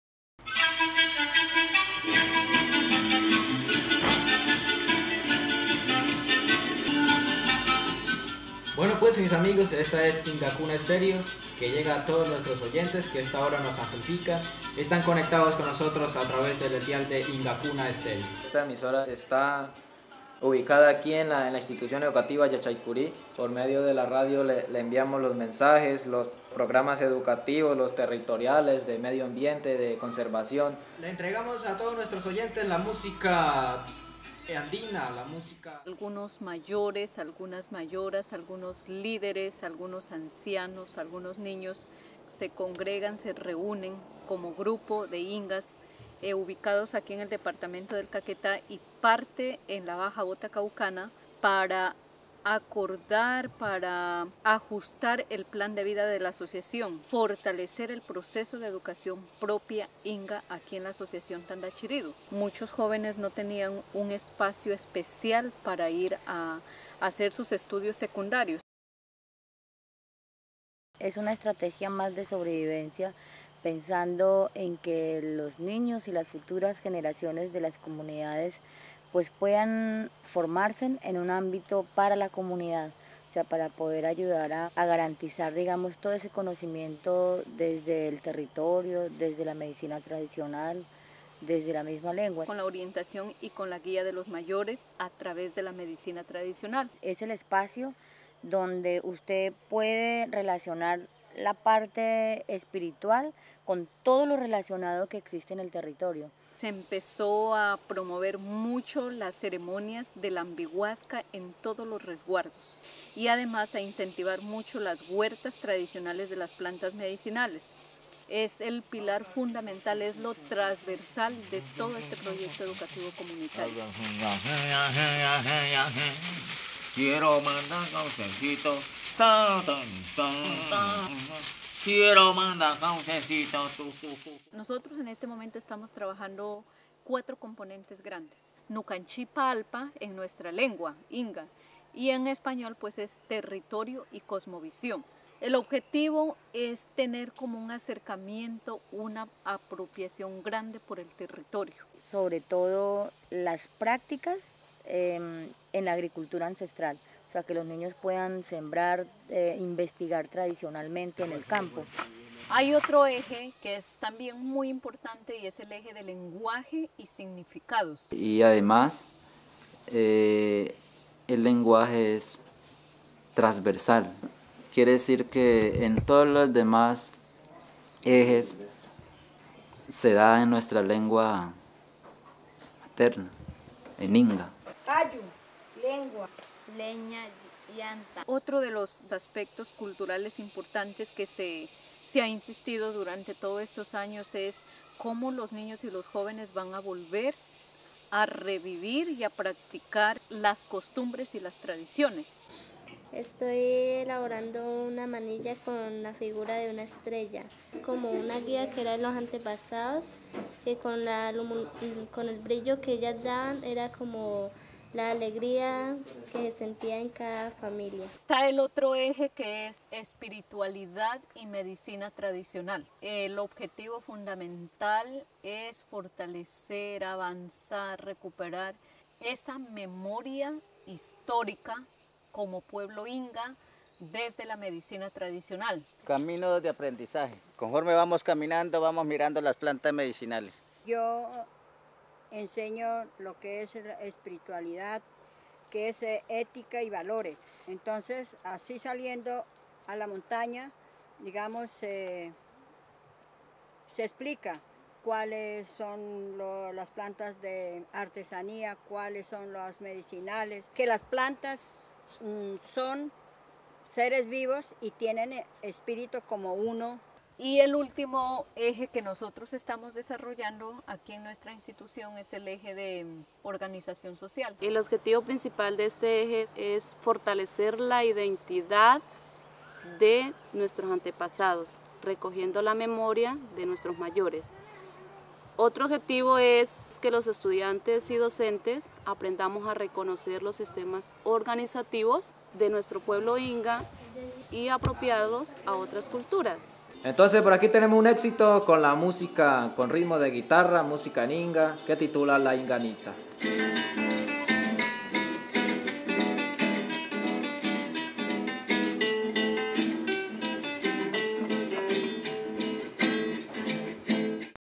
Camino al aprendizaje
This video describes the mission and history of the Yachaikury indigenous school in the Yurayaco indigenous reserve of Caquetá.

The school, founded in 2002 with the sponsorship of the Amazon Conservation Team (ACT), provides simultaneous instruction in traditional and standard western scholastic subjects.